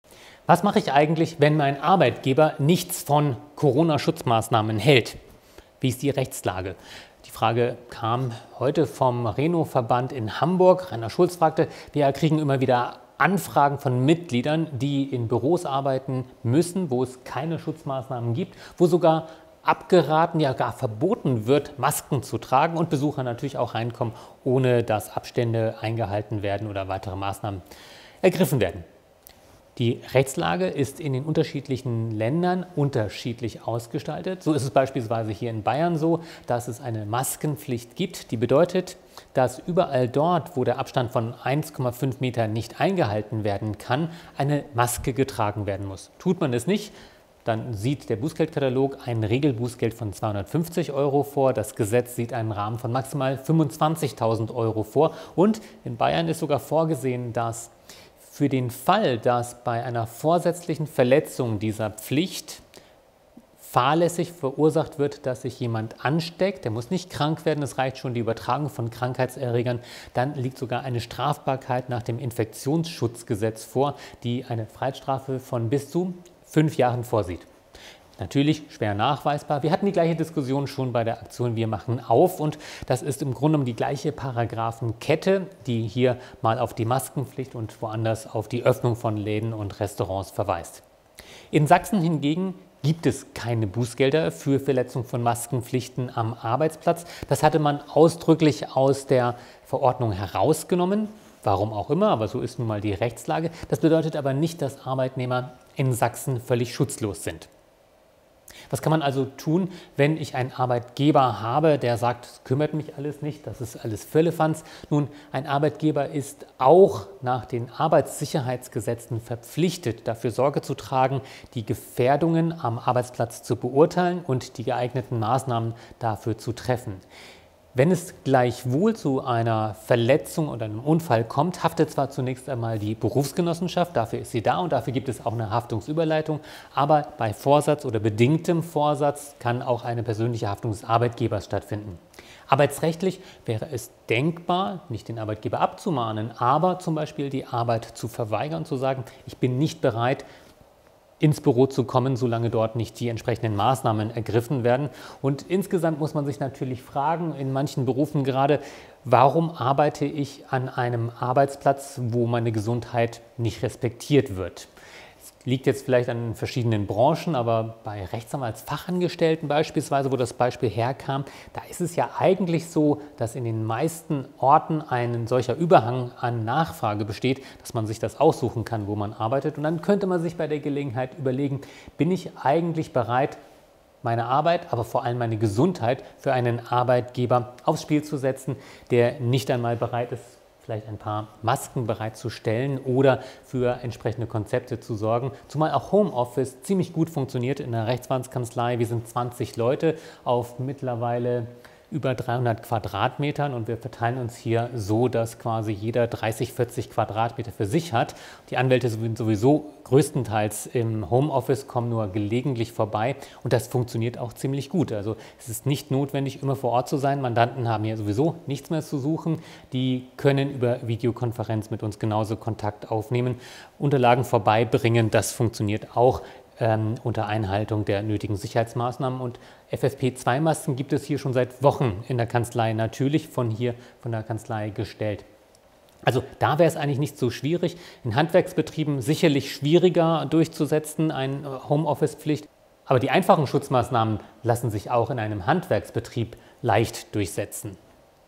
Hilfe - mein Chef verbietet mir, Maske zu tragen
Was kann ich tun, wenn mein Arbeitgeber Corona für ein Gerücht und Maßnahmen für Unfug hält? Betrachten wir die Rechtslage und die Handlungsmöglichkeiten.

Eine Rechtsanwaltsfachangestellte wendet sich an den Berufsverband:
Sehr geehrte Damen und Herren der ReNo Vereinigung,
so langsam wissen wir uns keinen Rat mehr. In unserer Kanzlei wird Corona keinerlei Beachtung geschenkt. Unser Chef sagt "er habe keine Angst davor" und es sei alles Quatsch was da so erzählt werde. Bei uns gibt es keinerlei Schutzmaßnahmen. Wir arbeiten zu dritt auf engem Raum. Keine Masken, keine Abtrennungen, Abstand halten nicht möglich. Zwar wäre Homeoffice durchaus möglich, darf aber nicht gemacht werden. Es besteht Anwesenheitspflicht, da wir für den Mandantenverkehr durchweg da sein müssen. Bei uns geht jeder ein und aus, ohne Masken. Kommen Mandanten mit Maske zur Besprechung wird seitens des Anwaltes gesagt: "Die brauchen sie hier bei uns nicht. " Ein Schild an die Eingangstür, dass Maskenpflicht besteht, dürfen wir nicht aufhängen. Haben wir denn als Refas überhaupt keine Rechte mehr und müssen und aufgrund von Querdenkermentalität diesem Riskio tagtäglich aussetzen?

Die Rechtslage ist in den Bundesländern unterschiedlich. Sachsen hat zwar eine Pflicht für Hygienekonzepte am Arbeitsplatz, einen Verstoß jedoch aus den Bußgeldern ausgenommen - warum auch immer. Die Pflicht, für Sicherheit zu sorgen, ergibt sich jedoch auch aus dem Arbeitsschutzrecht und der Fürsorgepflicht aus dem Arbeitsvertrag. Man muss sich am Ende aber als begehrte Fachkraft fragen: Warum arbeite ich für einen Arbeitgeber, der weder meine Arbeit noch meine Gesundheit respektiert, wenn anderswo Rechtsanwaltsfachangestellte händeringend gesucht und geschätzt werden?